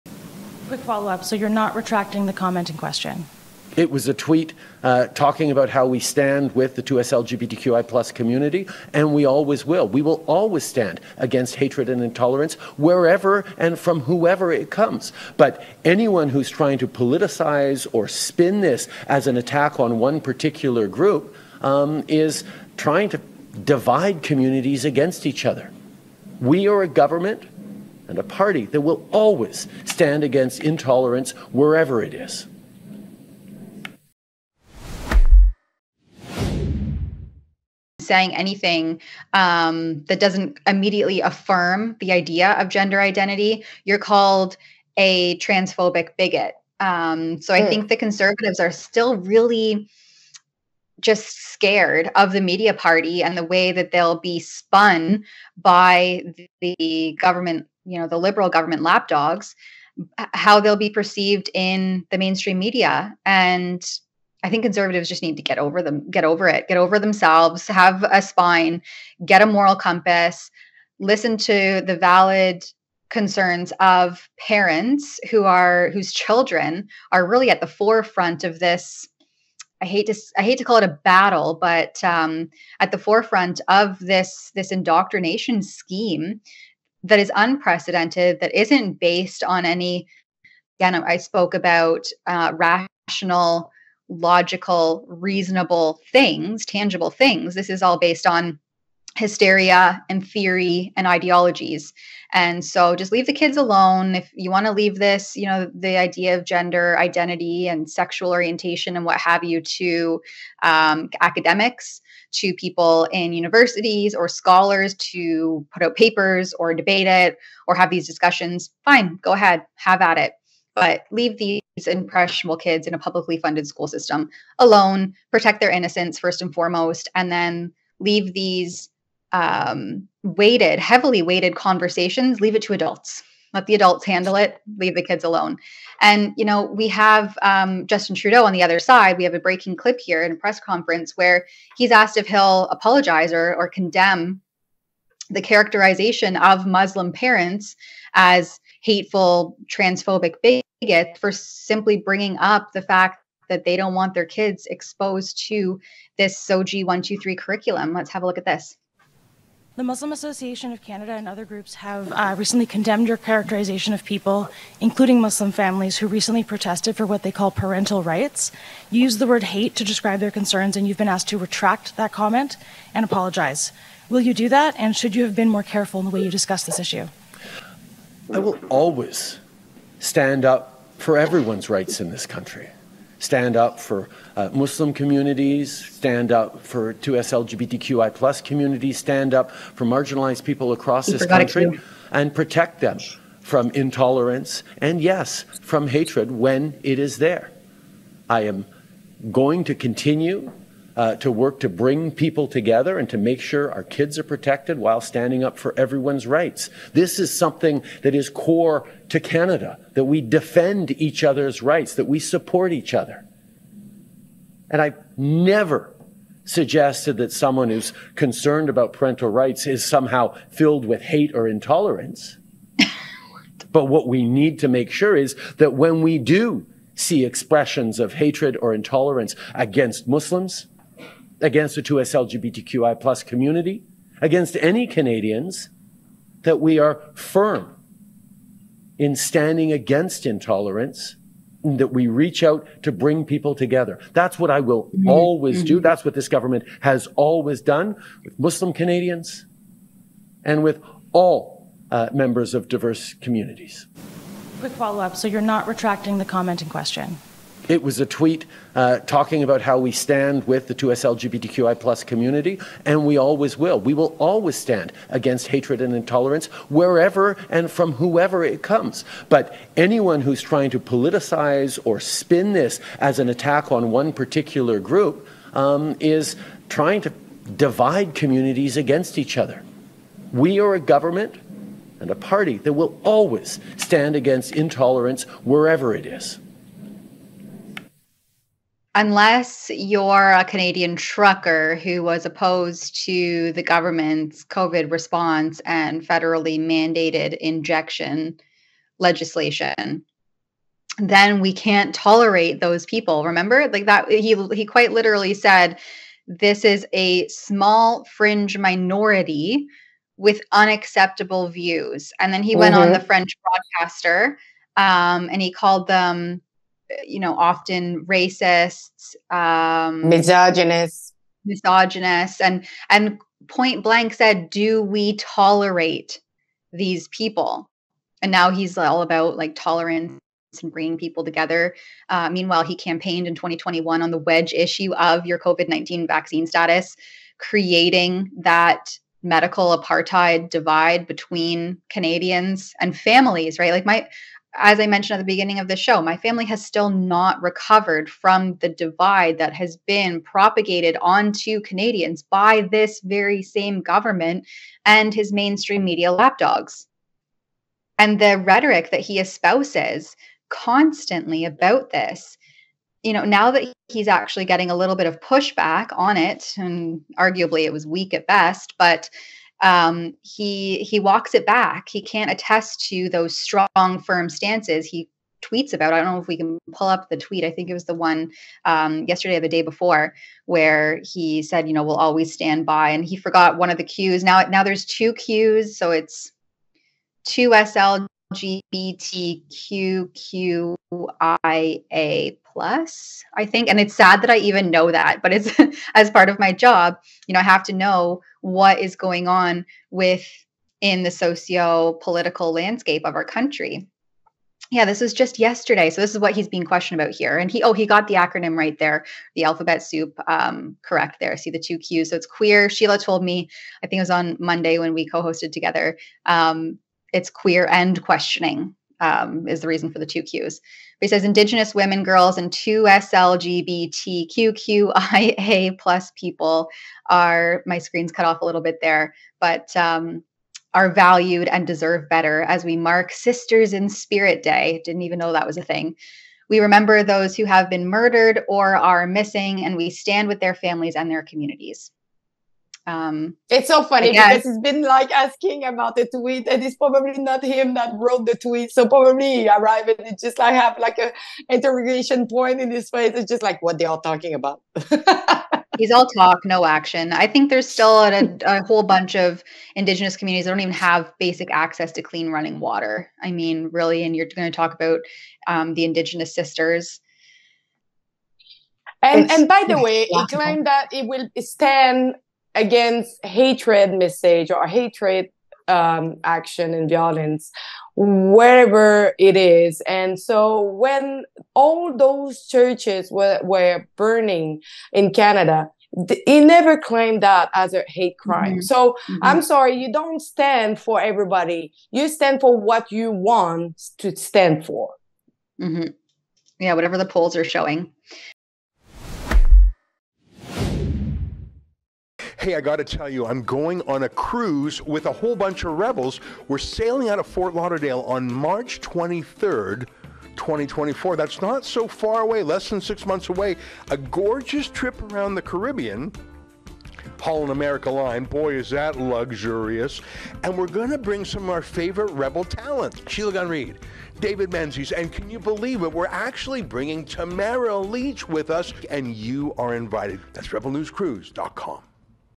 Trudeau refuses to apologize for calling Muslim protesters 'hateful'
| Trudeau refused to apologize for calling the Muslim community hateful, instead he came out with a statement that said, “I've never suggested that someone who is concerned about parental rights is somehow filled with hate or intolerance.”
“We are a government and a party that will always stand against intolerance wherever it is,” he added.
Tamara reacted to this clip and said: “Meanwhile, he campaigned in 2021 on the wedge issue of your COVID-19 vaccine status. Creating that medical apartheid divide between Canadians and families. My family has still not recovered from the divide that has been propagated on to Canadians by this very same government and his mainstream media lapdogs.”